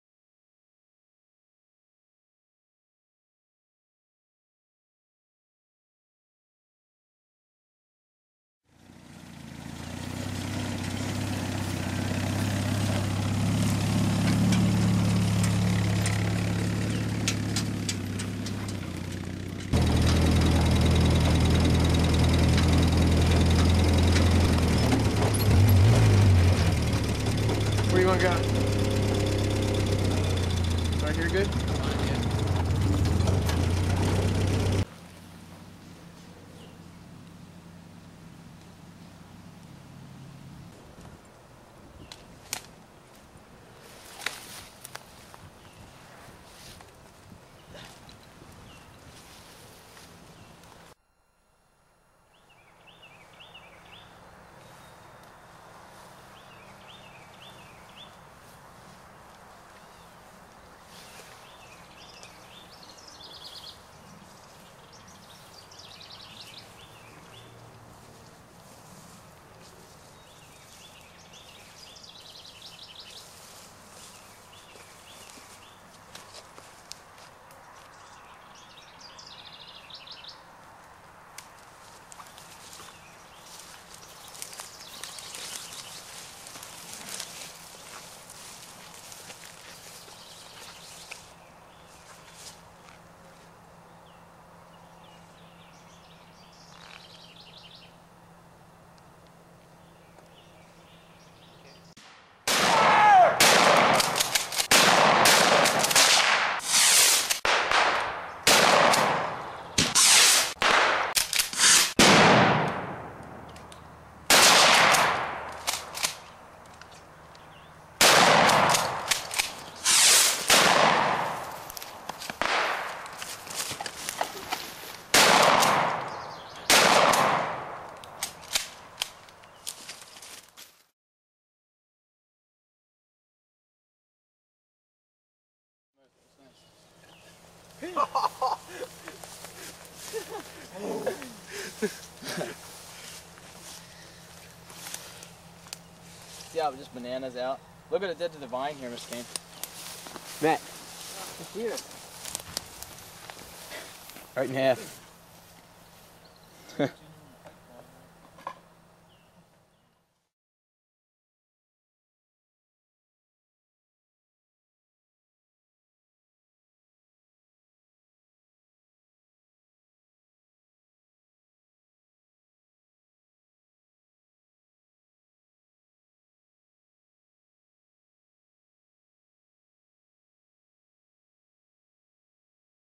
The Groundhog Wars
The country is being overtaken by groundhogs.  A determined band of patriots determine to resist the onslaught.  Watch as the epic battle for the survival of the species is joined.